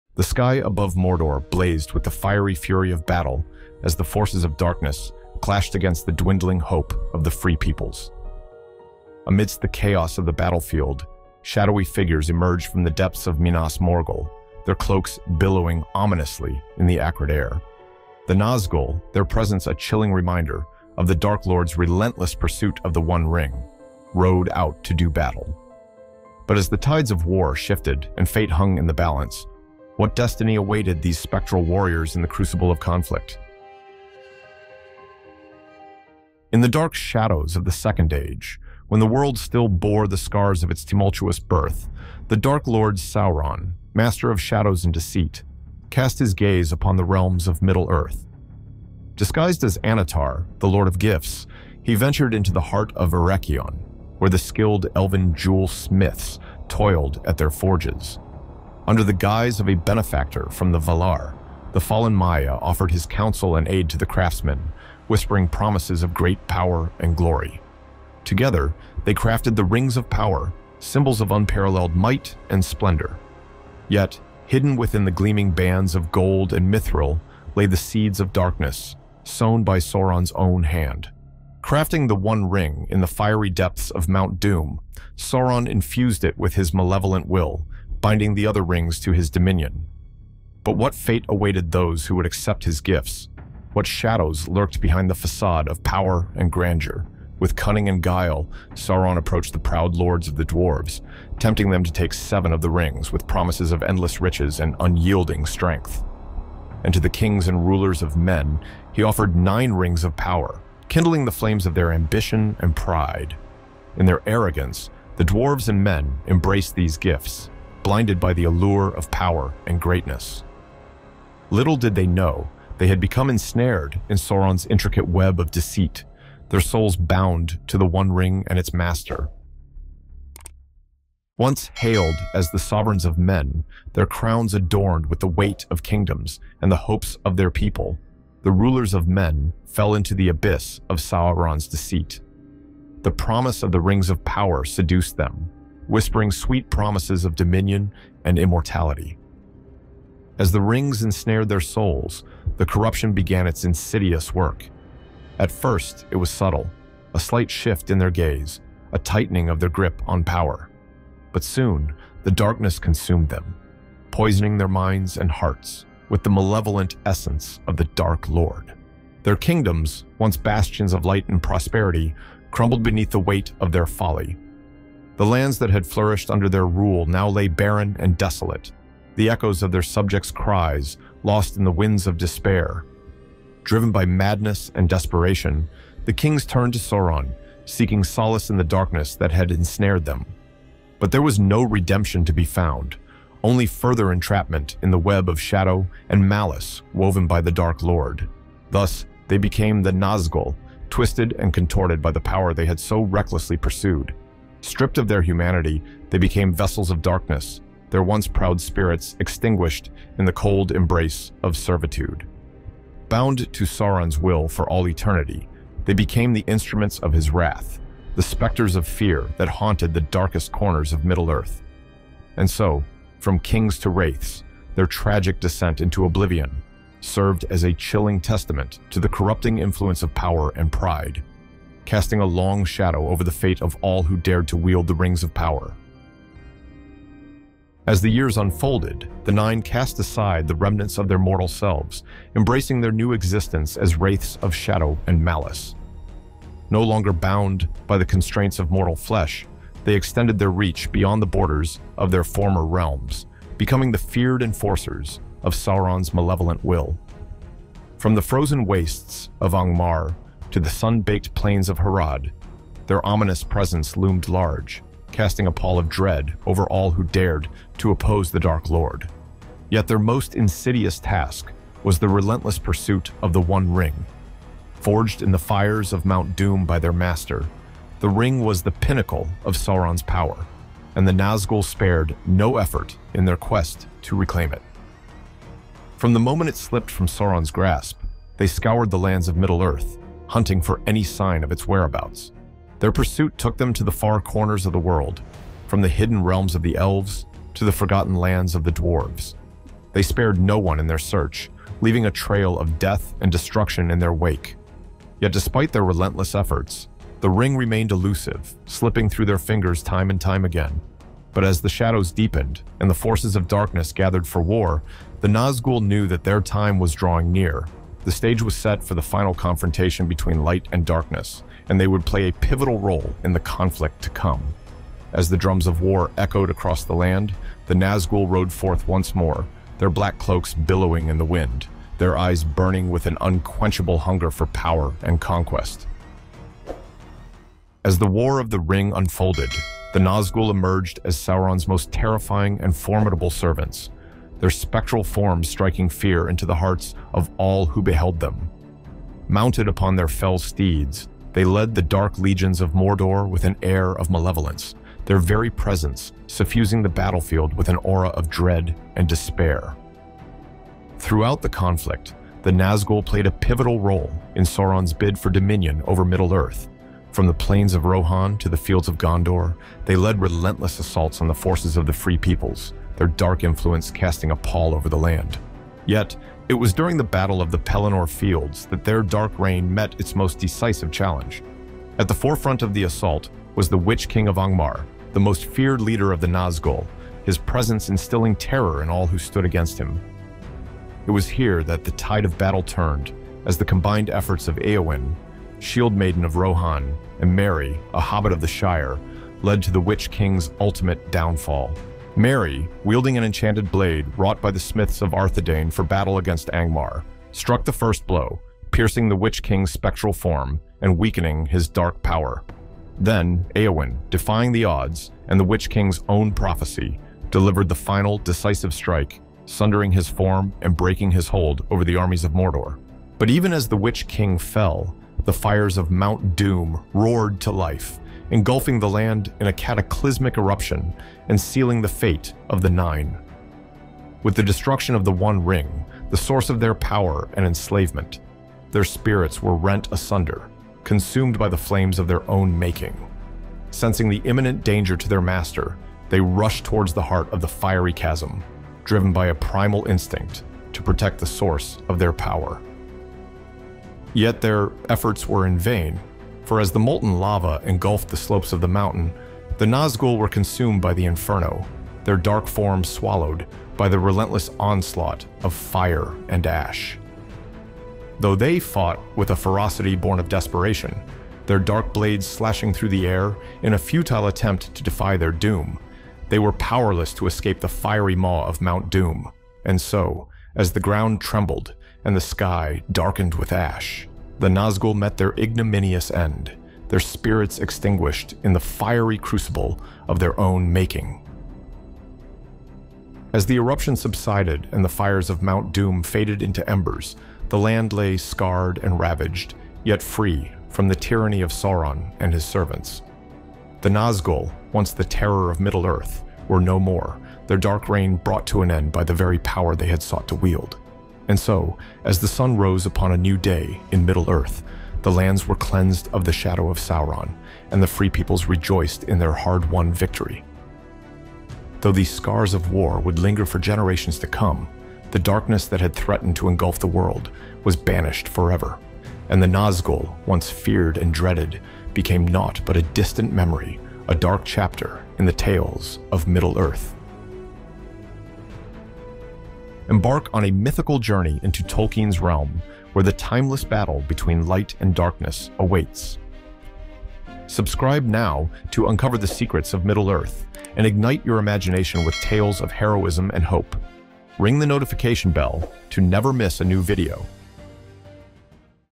The Most Ironic Decision That Led to the Ringwraiths Doom
This is the chilling tale of the Nazgûl during the War of the Ring in this amazing narrative story. Witness their emergence from Minas Morgul and learn of their tragic descent into darkness, ensnared by Sauron's cunning. Through the ages, these spectral warriors led a relentless pursuit of the One Ring, casting fear across Middle-earth. Yet, in their ultimate battle, against the backdrop of Mount Doom's eruption, their dark reign met its fiery end. Join us on a journey through the tumultuous history of the Nine and discover the fate that befell these fearsome servants of Sauron.

◽ Awesome Lord of the Ring FUNKO's
◽ ALL Legendary books by Tolkien
◽ DO NOT miss this Tolkien merchandise